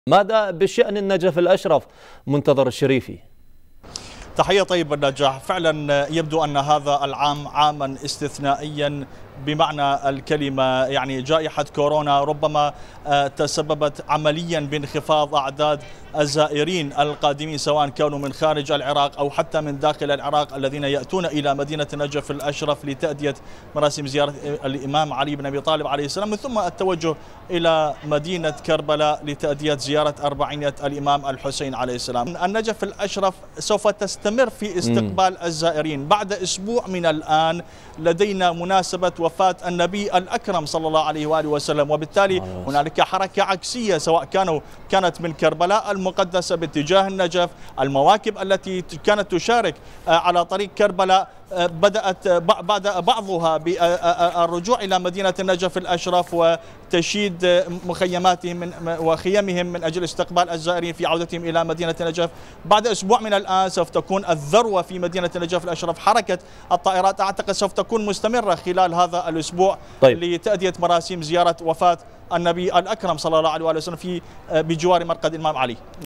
بعد إنتهاء زيارة الأربعين تستعد مدينة النجف لإستقبال الزائرين لإحياء ذكرى وفاة الرسول الأعظم 28 صفر
نشرة الساعة 12 ظهراً مع نجاح المولى
مراسل العراقية منتظر الشريفي